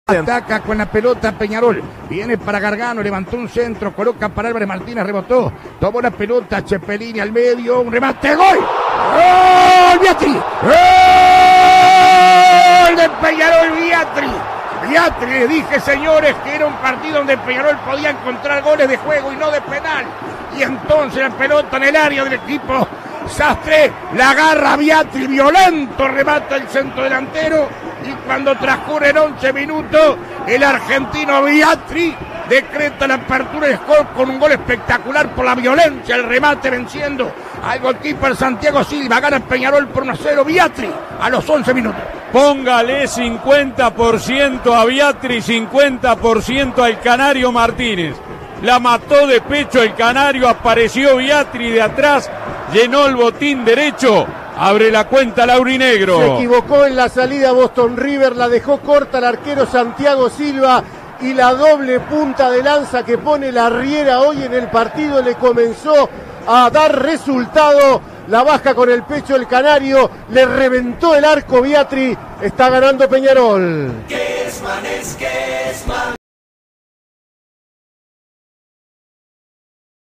Peñarol 1 Boston River 0 - Apertura 2022 (por Alberto Kesman)
El gol de Viatri, por Alberto Kesman junto a Richard Arce y Gustavo Ricci para CX 22 Radio Universal 970 AM.